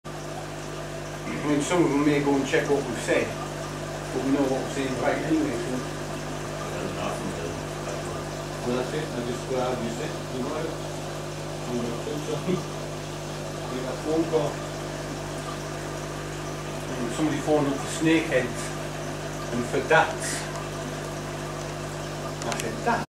X ray tetra at Tyne Valley Aquatics (near Newcastle)
Large X-ray tetra (Pristella maxillaris) at Tyne Valley Aquatics - top shop for tropical fish in north east England.